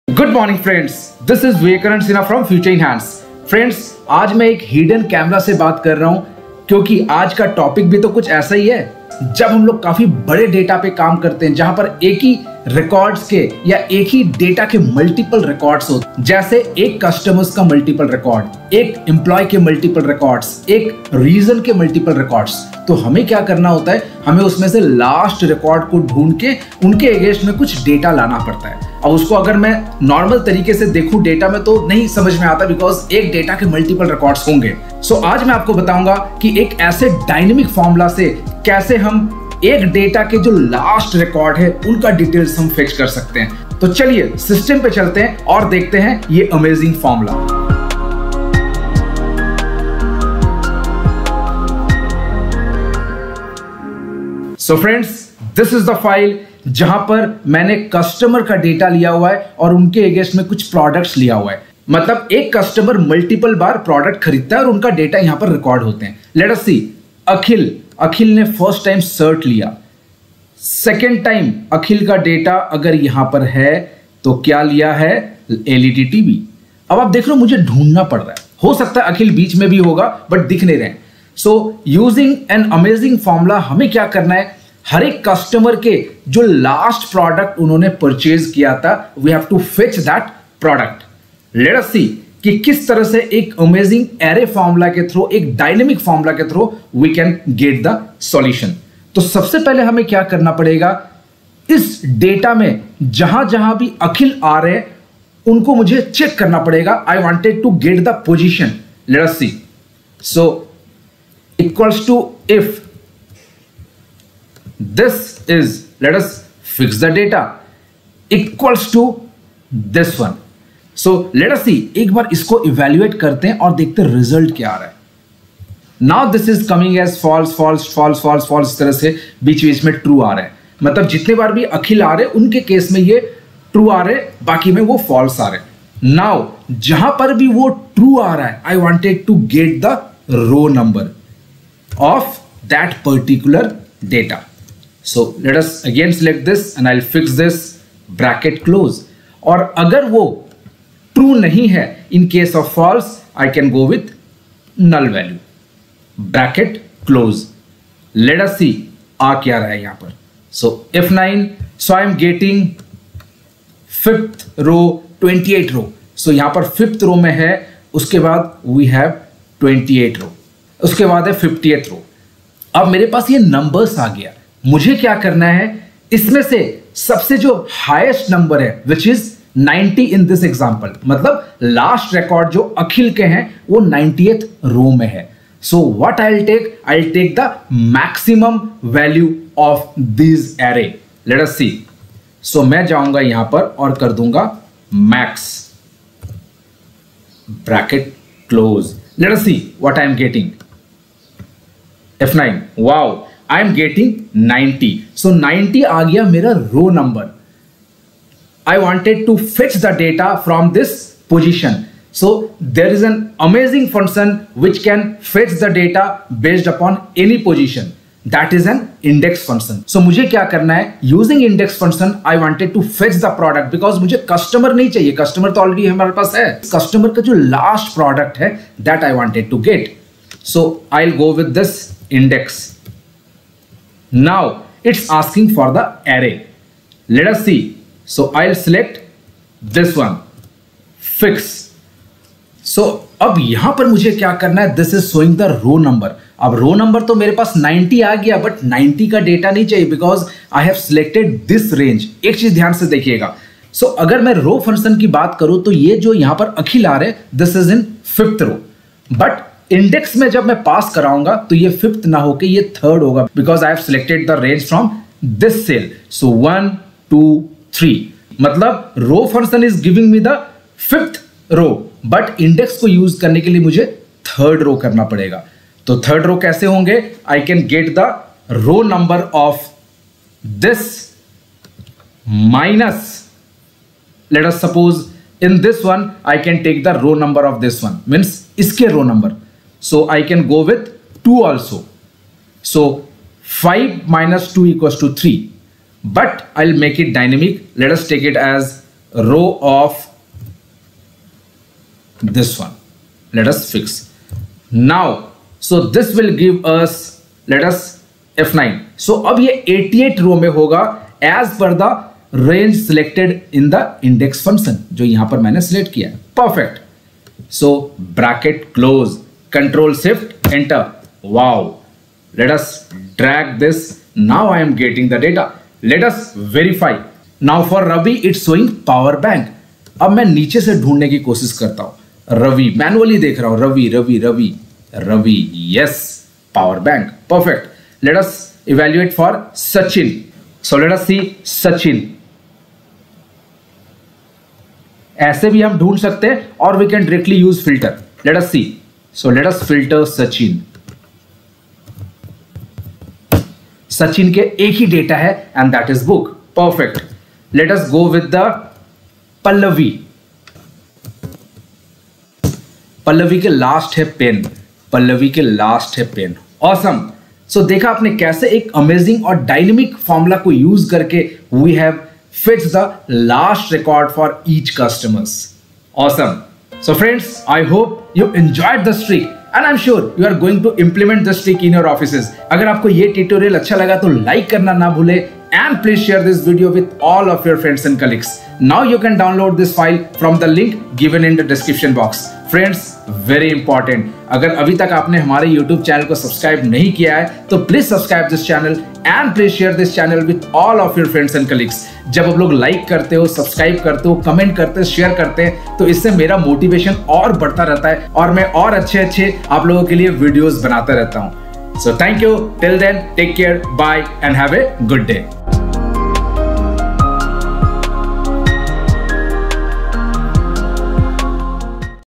Fetch Last Record in Excel (LOOKUP Last Record) | Vivekananda Sinha | Video 206
Fetch Last Record in Excel (LOOKUP Last Record)

While working on large data we often require the last record based on certain condition. Be it customer last purchase or employee last leave or any such data, its regular requirement that we have. Excel is all about wonders and when it comes to Array Formulas, then we can create any advance level of calculation. In this tutorial, I will show one amazing use of this. I will show how to fetch last record based on any condition. It will work across all versions of Excel. Be it Excel 2007, Excel 2010, Excel 2013, Excel 2016, Excel 2019, Excel 365.

In this Excel tutorial we cover a few important topics rather quickly:
Lookup last Record in Excel
Extract Data using Excel Array Formulas
Excel Array Formulas
Excel INDEX Function
Excel Nested Formulas